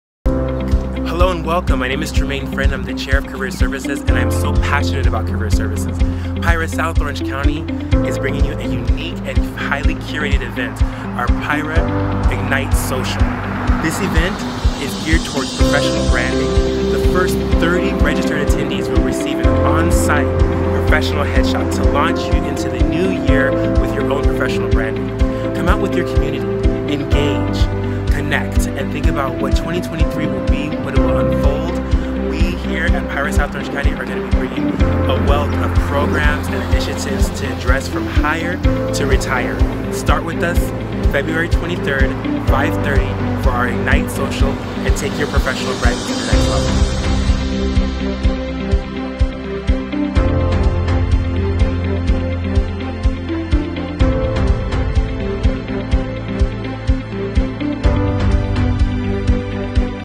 Pihra South Orange County Career Services Ignite Event - 2023
Ignite your professional brand and career development with PIHRA South OC Career Services.

PIHRA – South OC Career Services is excited to host the Career Services
Ignite Social! This social event is focused on igniting awareness of Career Services & Emerging Leaders, uniting our HR community, and inspiring a fresh perspective on professional branding and career development.

Join us to ignite your unique professional brand and community engagement! The first 30 registered attendees will receive a scheduled on-site professional headshot provided by industry leaders The Headshott Truck | Capturely.